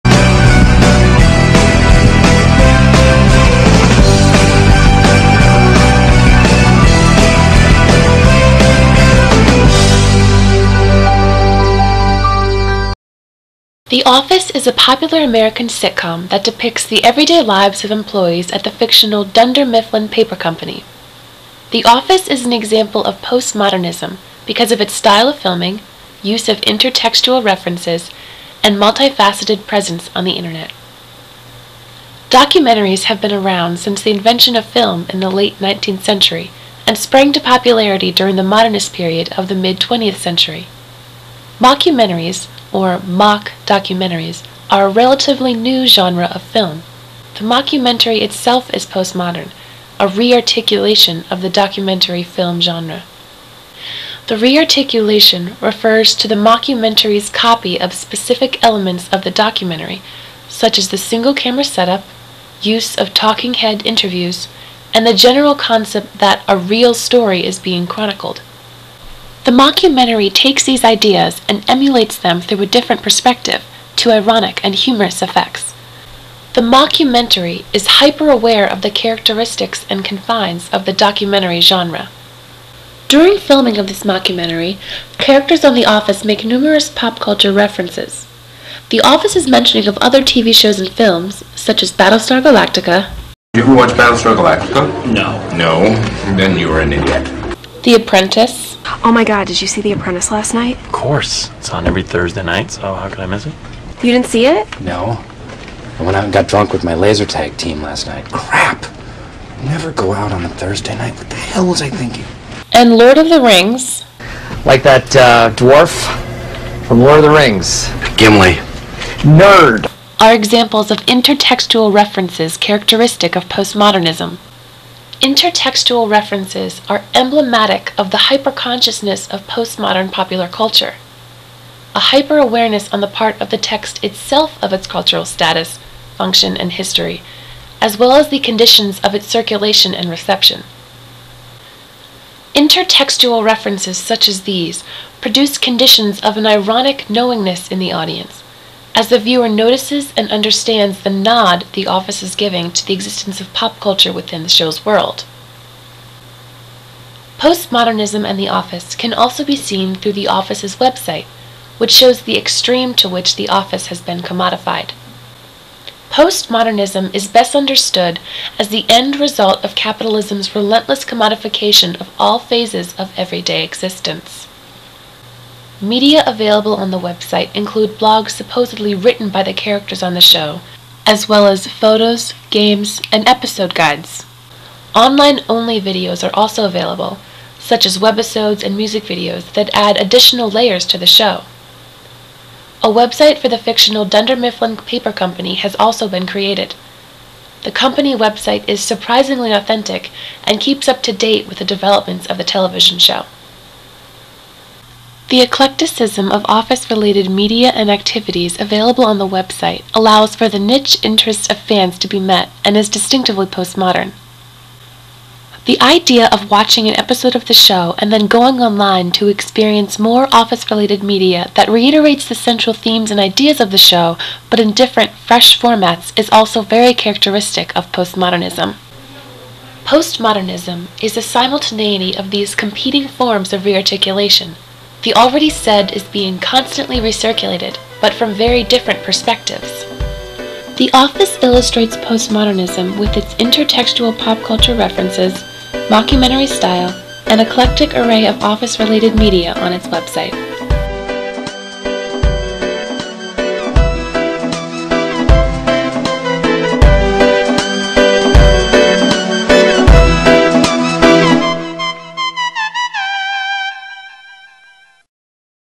Postmodernism and The Office
Explanation of Postmoderism and how it relates to The Office.

MCS 333 Final Project